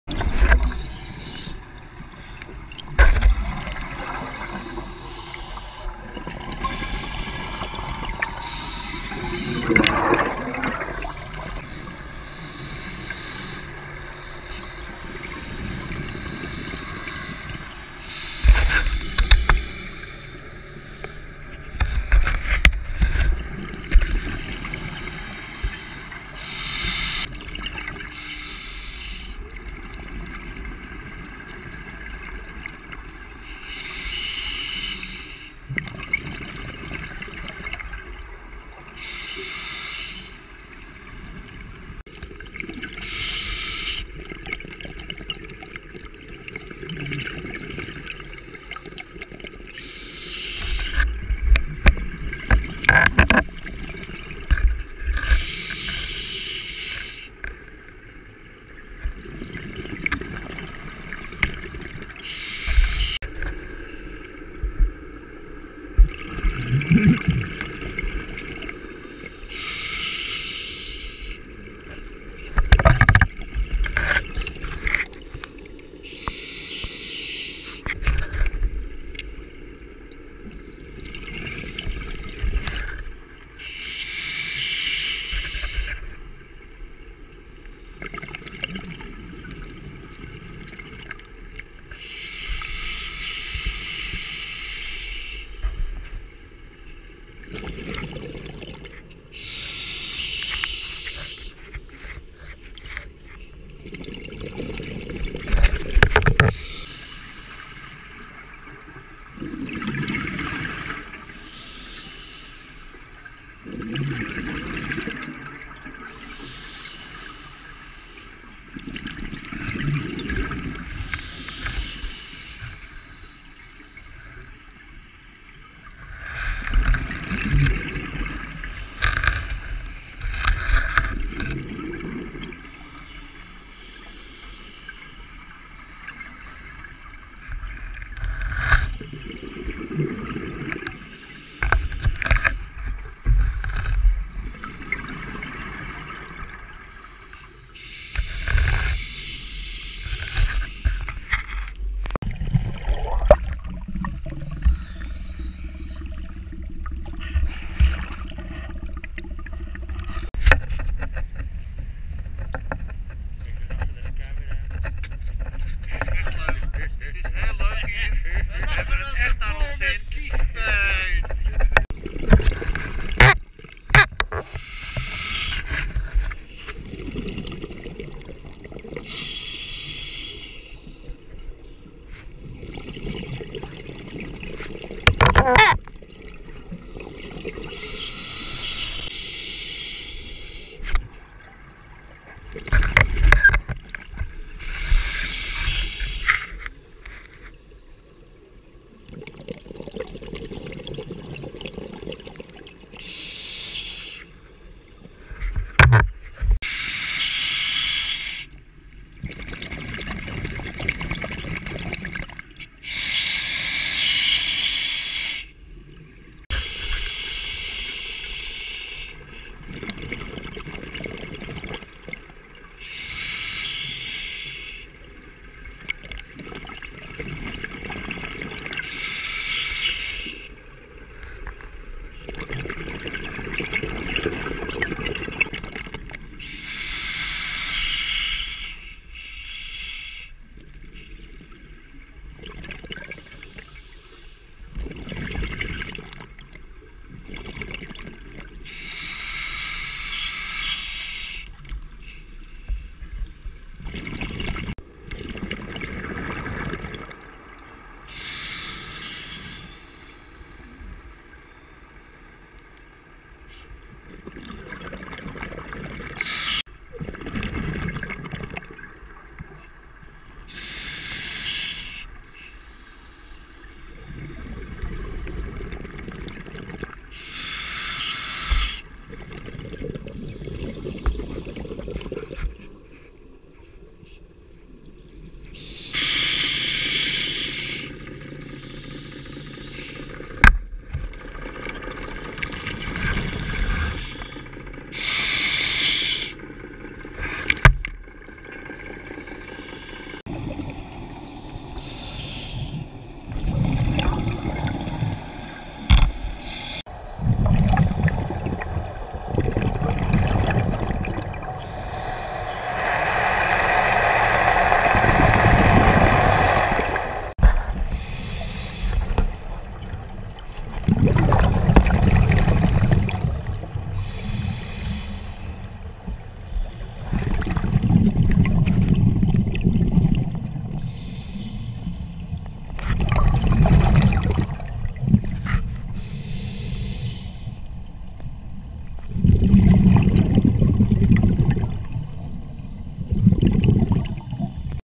Wrakduik 24 sep 2017 HMS Cressy en HMS Aboukir
Nitrox dive on the ship wrecks Cressy and Aboukir. North Sea